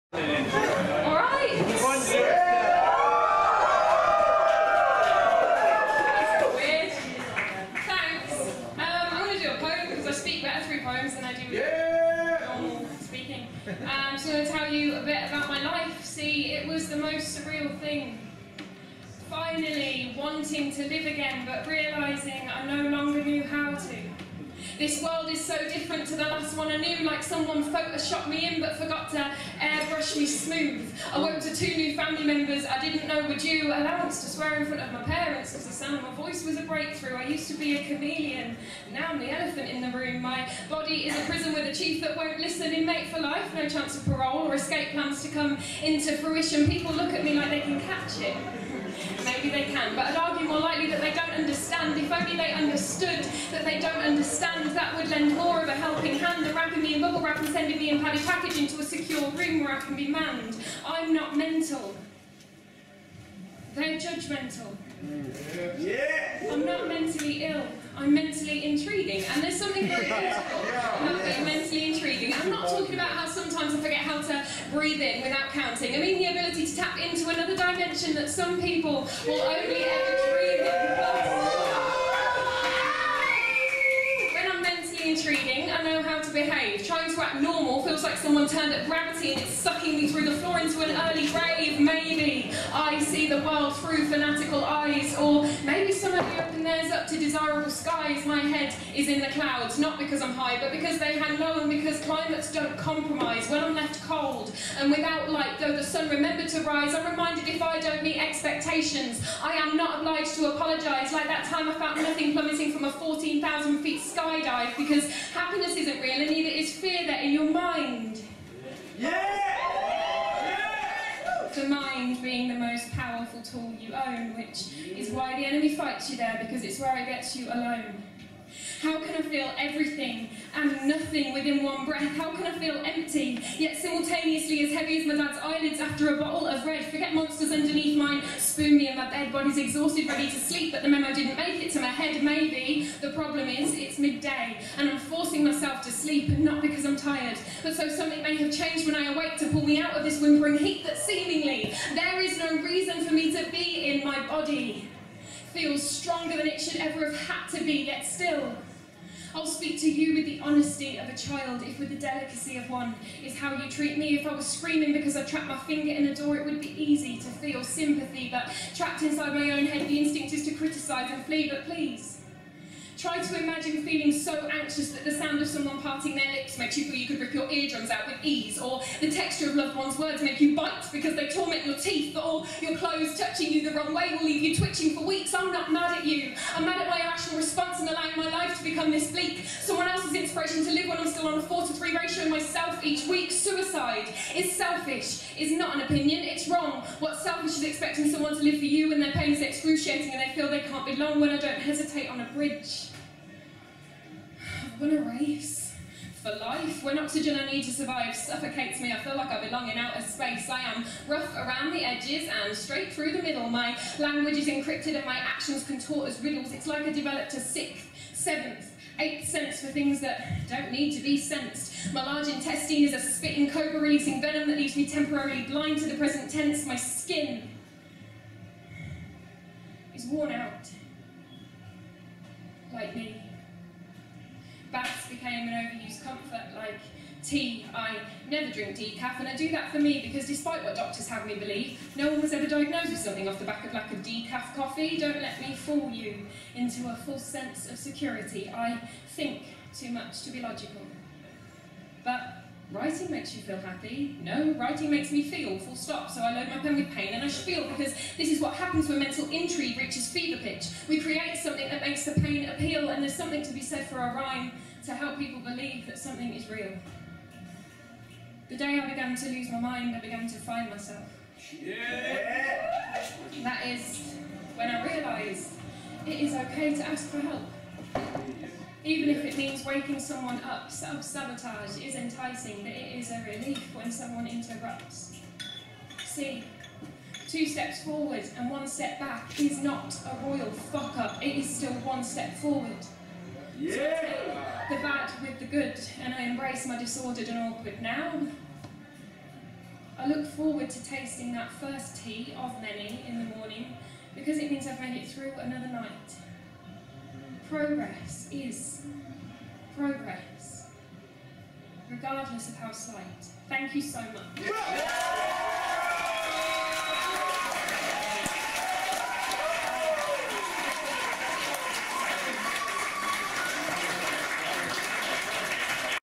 Jemima Hughes Slam Winner @Some-Antics 05/04/2019
Some-Antics is a spoken word event that takes place upstairs in Brew Dog, Leicester, on the first Friday of every month. It has three sections- A featured act, spoken word by local poets and a poetry slam. If you would like to get involved please let us know on the Facebook event page